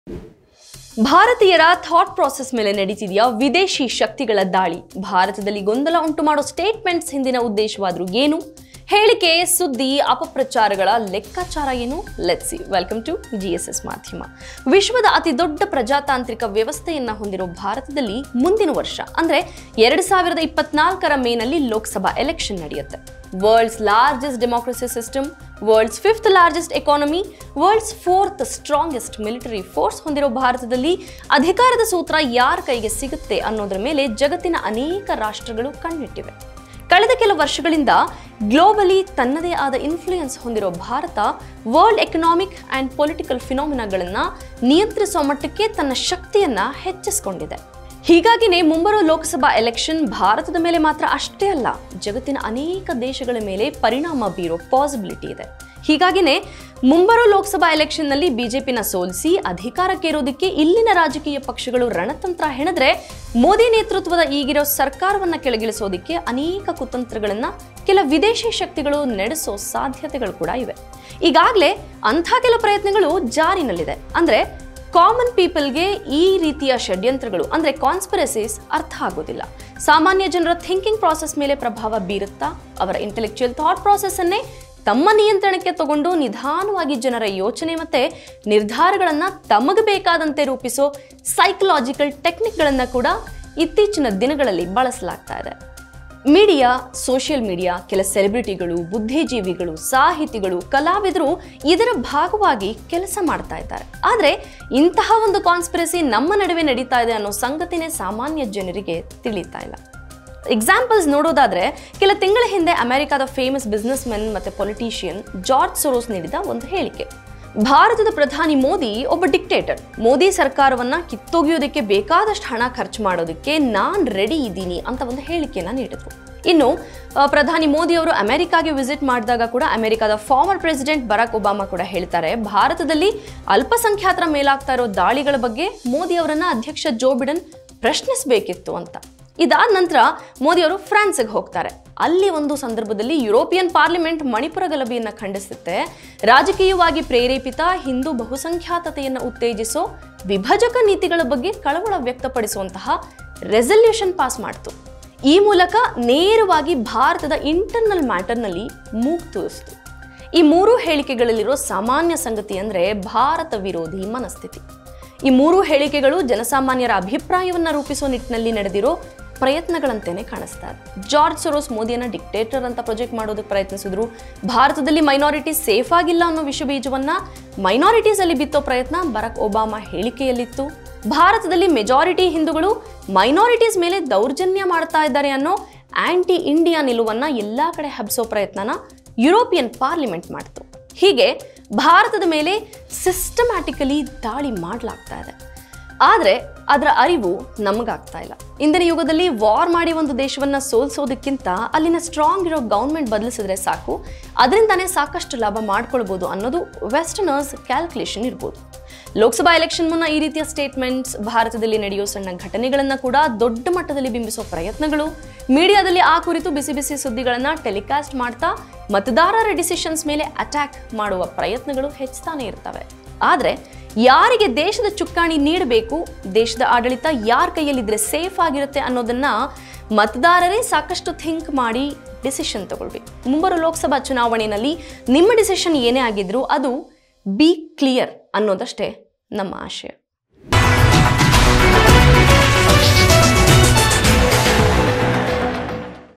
ಭಾರತದ ಚುನಾವಣೆಯಲ್ಲಿ ವಿದೇಶಿ ಶಕ್ತಿಗಳ Psychological techniques..! | Election 2024 | GSS MAADHYAMA
2024ರಲ್ಲಿ  ಭಾರತದಲ್ಲಿ  ನಡೆಯುವ   ಲೋಕಸಭಾ ಚುನಾವಣೆ  ಮೇಲೆ  ವಿದೇಶಿ ಶಕ್ತಿಗಳು ಬೀರುವ ಪ್ರಭಾವಗಳ ವಿವಿಧ ಆಯಾಮಗಳ ಕುರಿತು ಇಲ್ಲಿ ಚರ್ಚಿಸಲಾಗಿದೆ.

 Various dimensions of influence of foreign powers on the 2024 Lok Sabha elections in India are discussed here.

CONCEPT AND DIRECTED BY : C.K. MAHENDRA
D.O.P : NAVEEN KUMAR
ANCHOR : ROOPASHREE M R
CONTENT CREATOR : MAHESH HIREMATH
GRAPHICS & VIDEO EDITING : DAIVIK. R, MADHU. G.B
CO-ORDINATOR: SARALA J